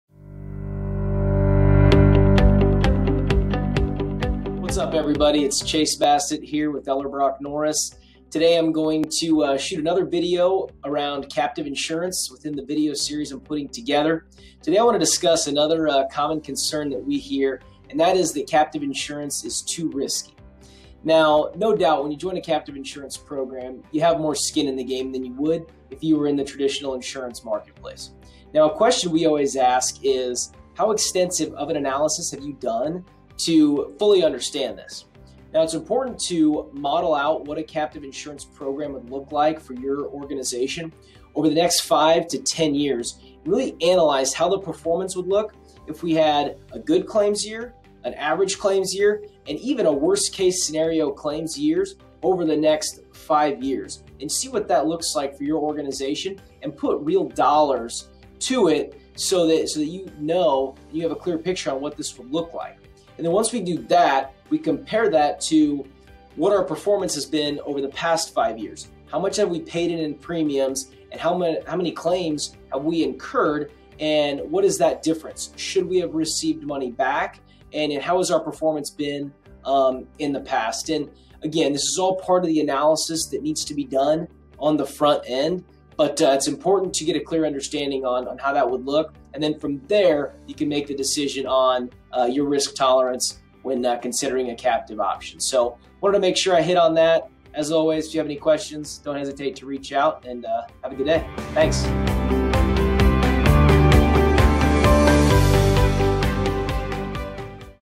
Common Concern #2: Too Risky? - Captive Insurance
Risk Advisor Chase Bassett discusses the concern of captive insurance being too risky. Find out his thoughts in this episode!